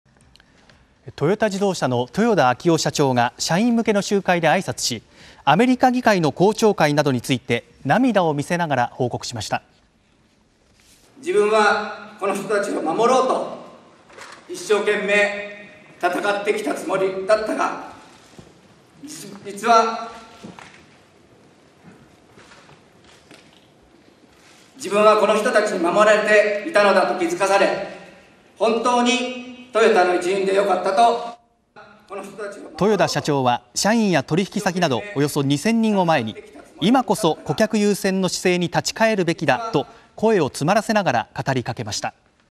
トヨタ豊田社長が社員集会で「公聴会」涙の報告（10/03/05）
トヨタ自動車の豊田章男社長が社員向けの集会で挨拶し、米議会の公聴会などについて涙を見せながら報告しました。